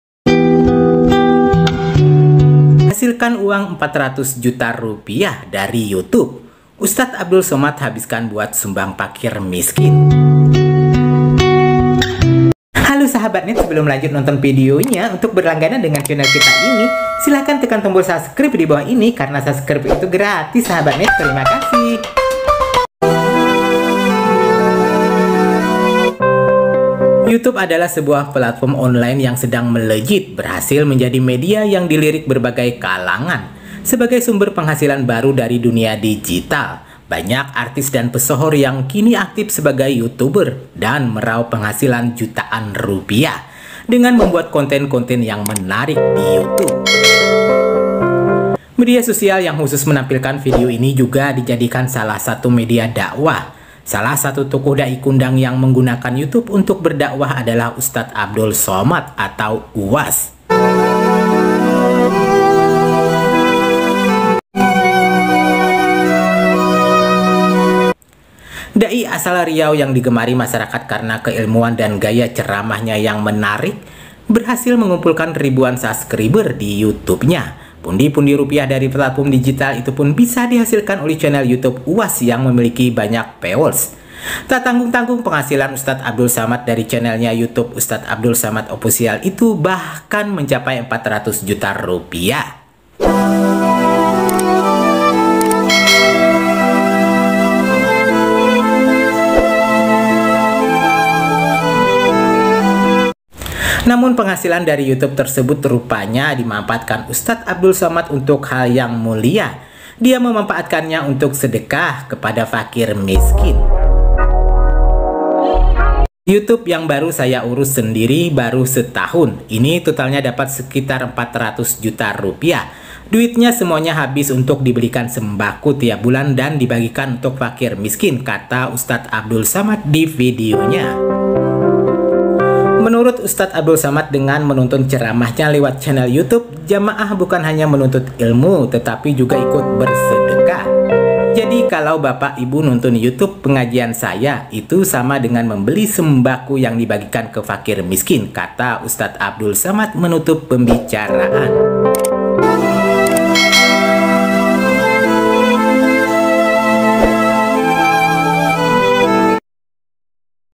Ustadz Abdul Somad Hasilkan Rp 400 juta dari YouTube! Dihabiskan Sumbang untuk Fakir miskin
Assalamualaikum Hallo sahabatnet apa kabar semoga hari ini menyenangkan ini kita ada video baru lagi nih yaitu.ustad Abdul Somad atau UAS hasilkan uang dari YouTube Rp400juta habis di sumbangkan untuk Fakir miskin dia belikan sembako dan Di bagikan ke warga yang membutuhkan.